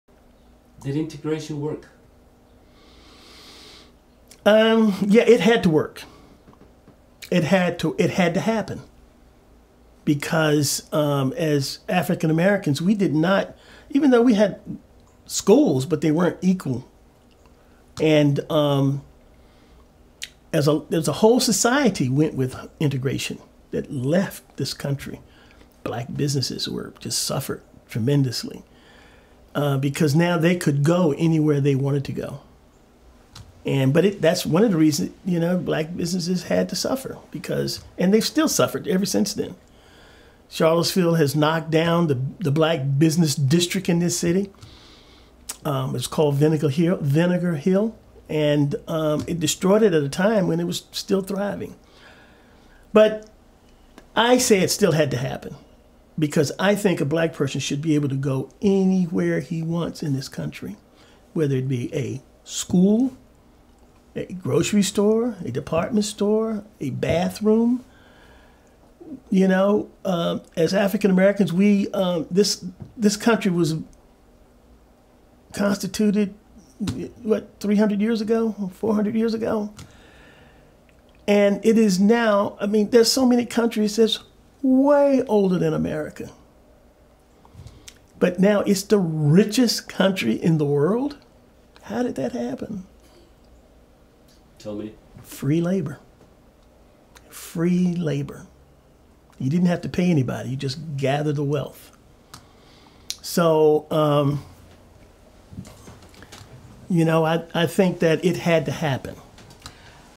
Ordinary Men | Robert King
Project Charlottesville: I recently had the opportunity to record my conversations with Mr. Robert King, of Charlottesville Virginia. Over several hours Mr. King shared his views on Integration, Institutional Racism and Race in America. This segment in particular focuses on part of his experience with regards to integration.